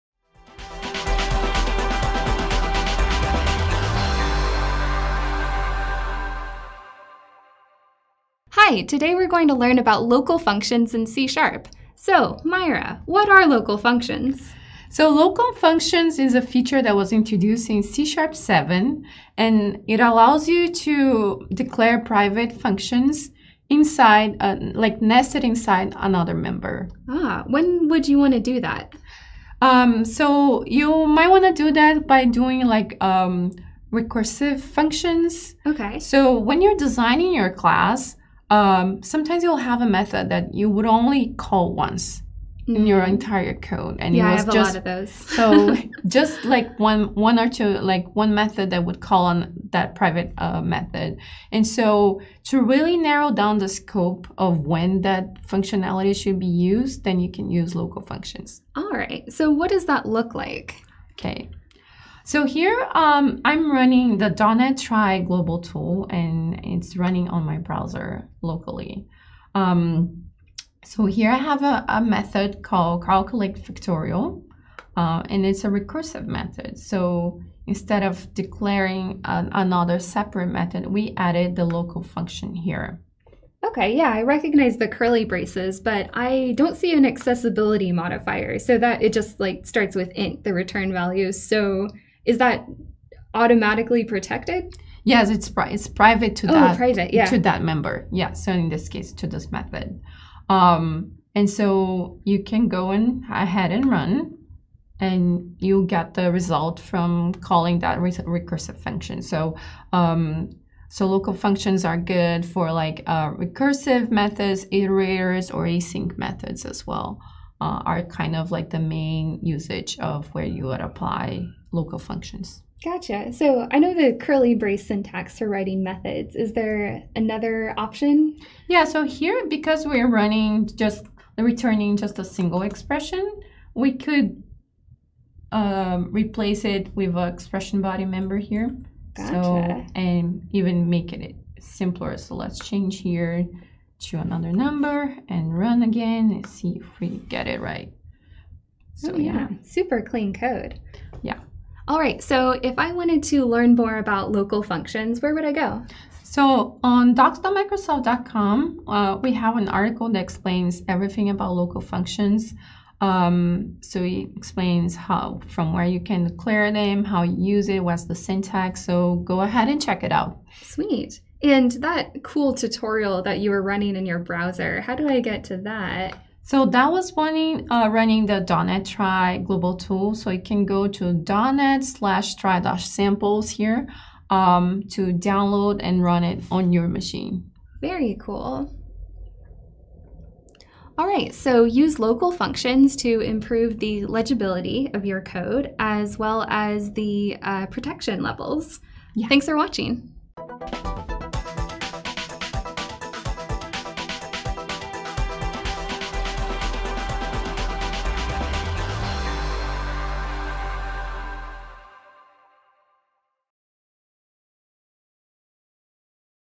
C# Language Highlights: Local Functions
In this short video, Maira and Kendra will show you how to use local functions in C#. To learn more about other C# features, head over

Useful Links